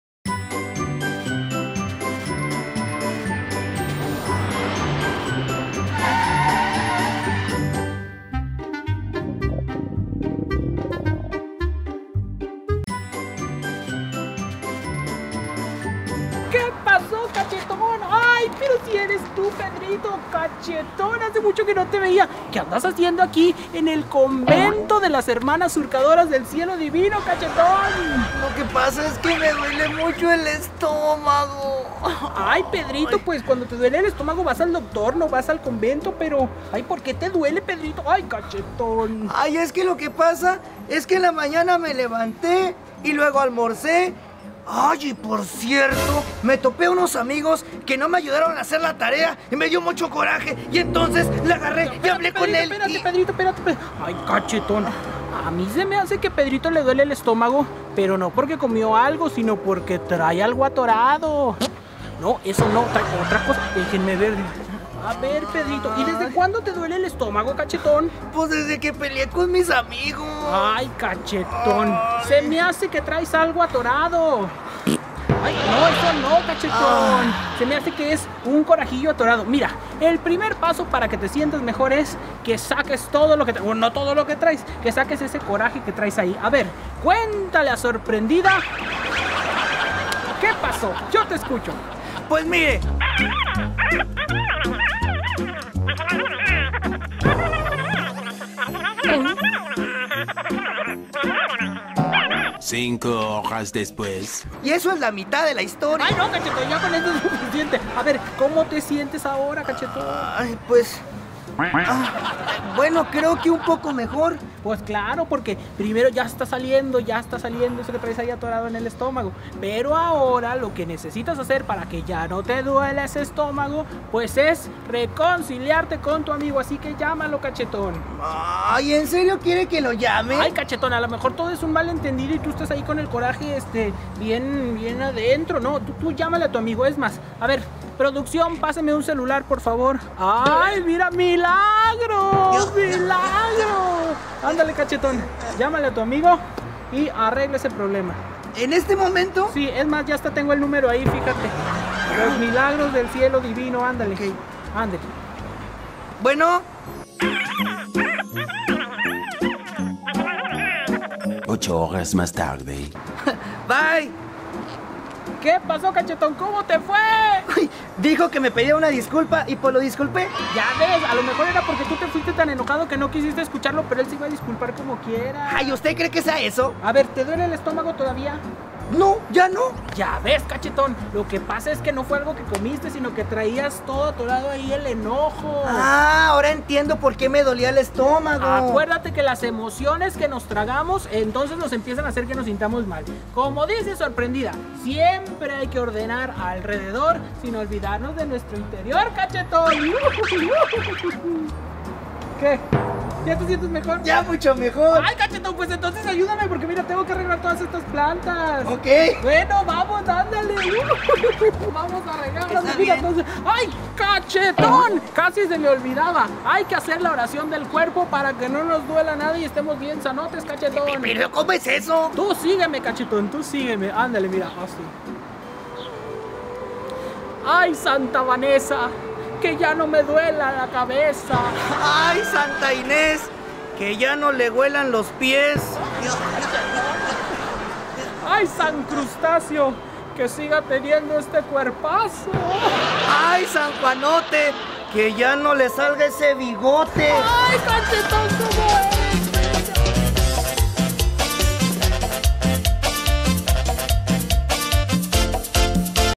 A Pedrito le duele el estómago | Jerry Ye
Pedrito visitó a Sor Prendida porque le duele mucho el estómago y no sabe por qué.